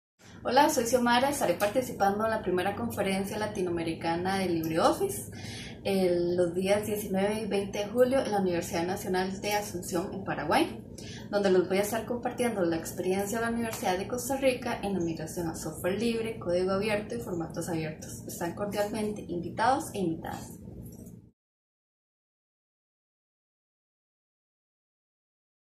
LibOCon Latinoamérica - Asunción 2019
English (subtitles available): The LibreOffice Latin America Conference 2019, will be held at the Facultad Politécnica de Universidad Nactional de Asunción (FPUNA) in Asunción, Paraguay on July 19th (Friday) and 20th (Sat).

LibreOffice Latin America Conference will be the first event gathering LibreOffice users, advocates and contributors (not only development, but also localization, PR/marketing, documentation, quality assurance, … etc.) from different countries in Latin America, to exchange and share experiences and knowledge.

For more information about the conference please visit the official

Spanish: La Conferencia LibreOffice Latinoamérica 2019, se llevará a cabo en la Facultad Politécnica de Universidad Nactional de Asunción (FPUNA) en Asunción, Paraguay el 19 (viernes) y 20 de julio (sábado).

La Conferencia LibreOffice Latinoamérica será el primer evento que reunirá a usuarios, defensores y colaboradores de LibreOffice (no sólo de desarrollo, sino también de localización, relaciones públicas/marketing, documentación, control de calidad, etc.) de diferentes países de América Latina, para intercambiar y compartir experiencias y conocimientos.

Para obtener más información sobre la conferencia, visite el sitio web oficial